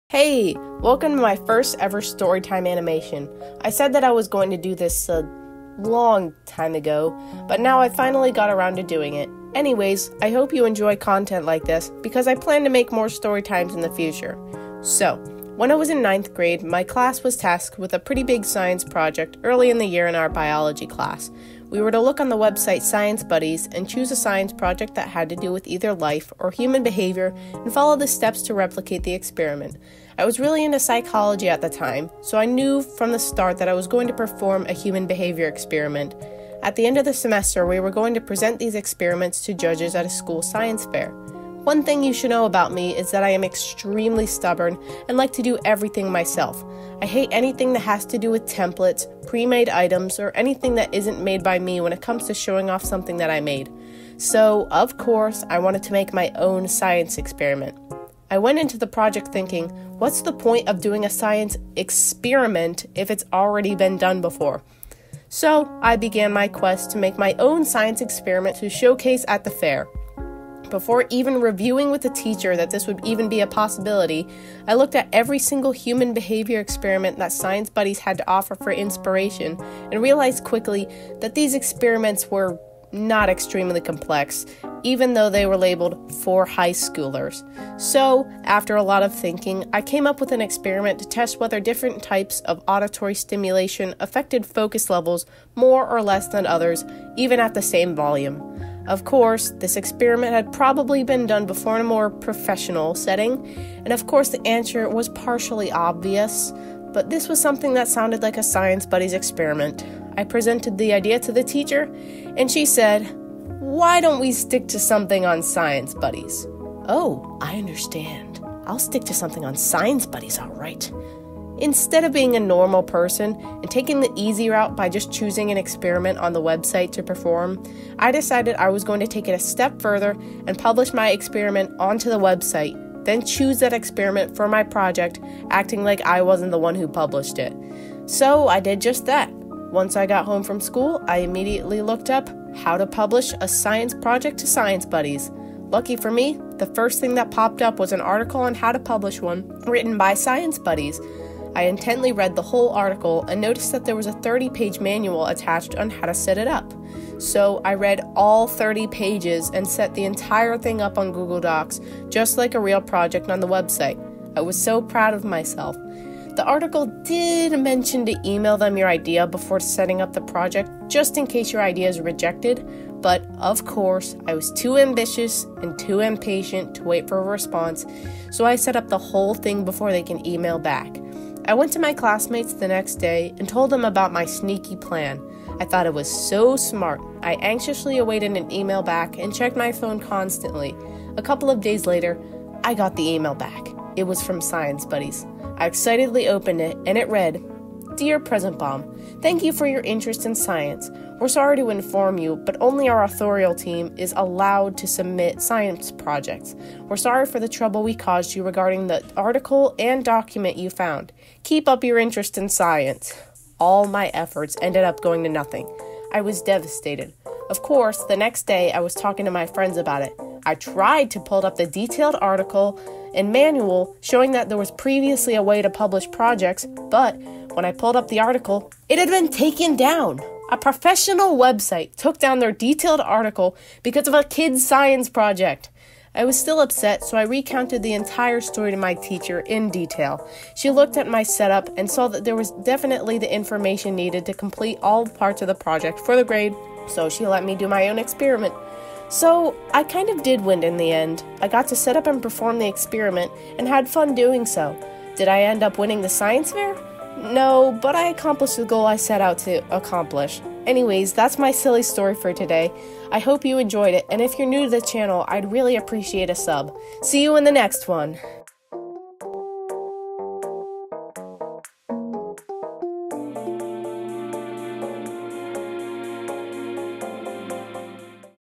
This website took down their article because of MY science Project!
Hey! Finally, my first storytime animation! I worked pretty hard on this, so I'd appreciate a sub! In this video I talk about my ridiculous science project attempt and how it lead to a science article being taken down! Hope you enjoy!